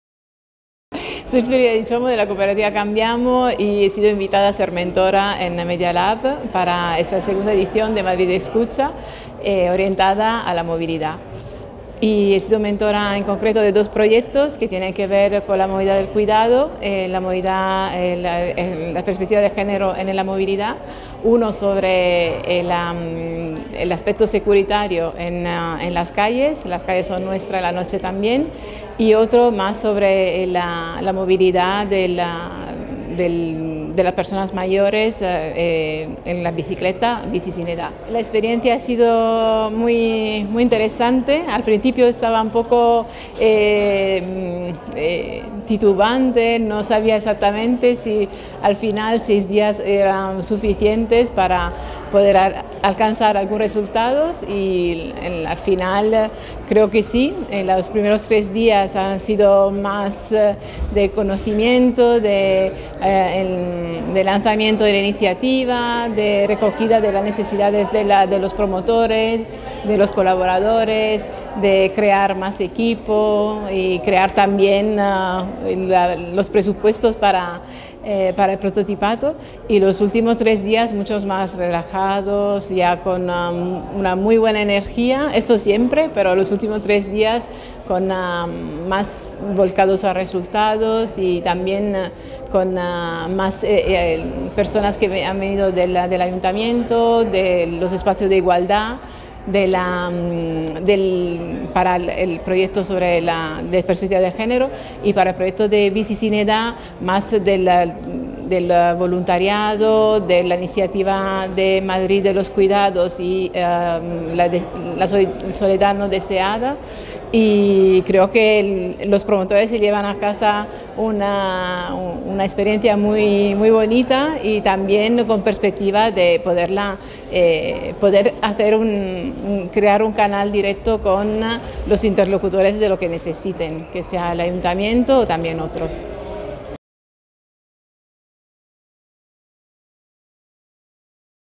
Entrevista a Floridea Di Ciommo. Madrid Escucha. Moverse en la ciudad.
Floridea Di Ciommo nos habla de su función como mentora y hace una valoración sobre este taller dedicado a la movilidad urbana.